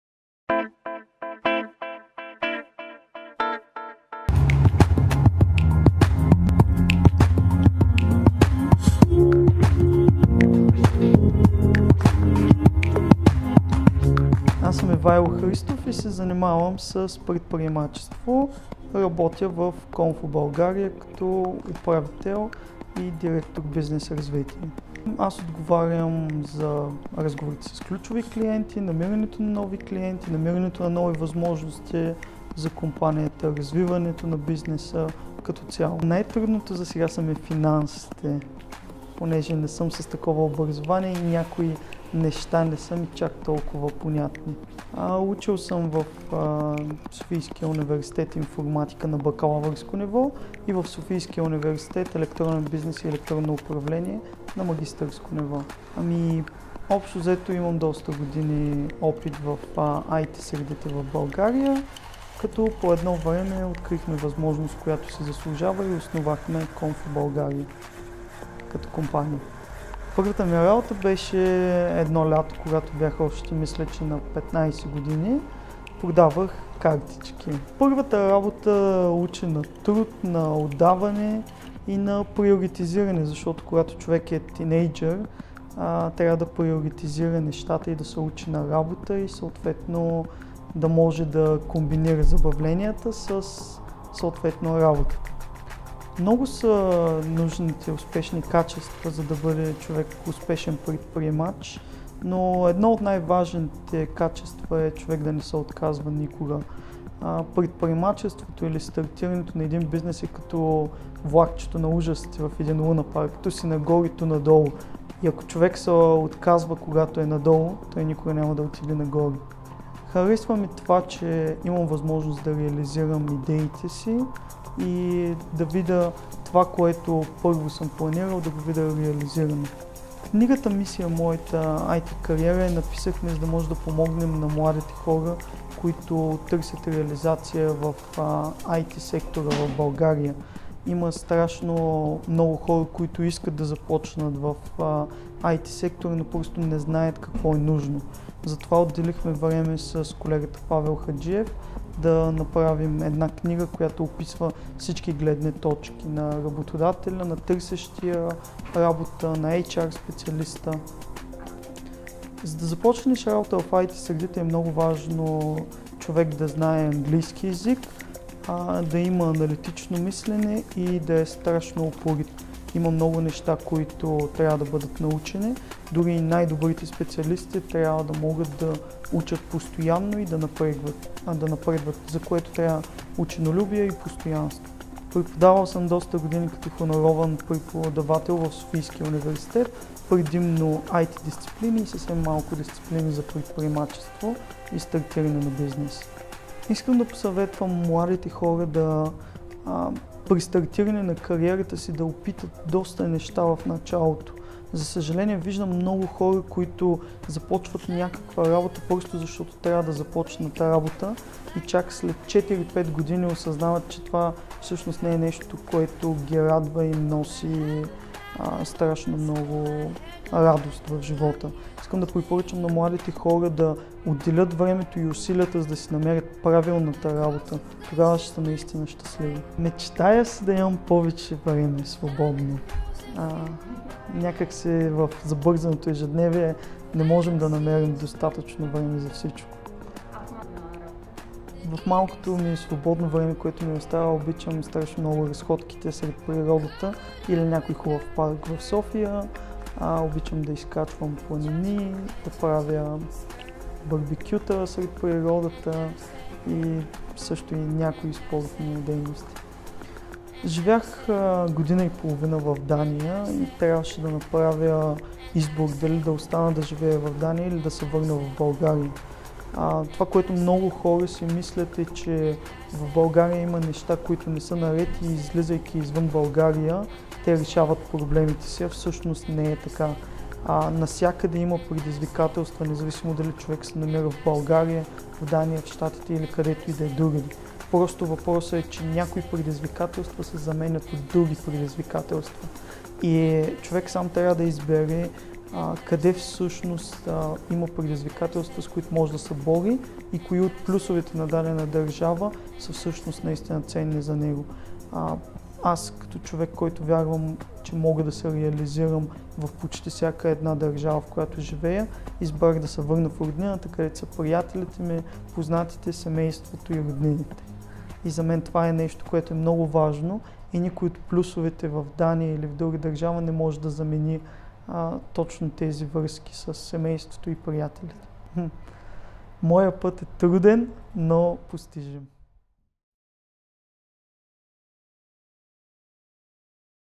Ивайло Христов - директор бизнес развитие на "Комфо България"
Искате да работите в сферата на високите технологии? Искате да надграждате върху съществуващите социални мрежи като Facebook? Разберете повече за това от г-н Ивайло Христов, съосновател и директор бизнес развитие на "Комфо България".

Допълнителна информация за твоето професионално и академично развитие можеш да намериш тук: wishbox.org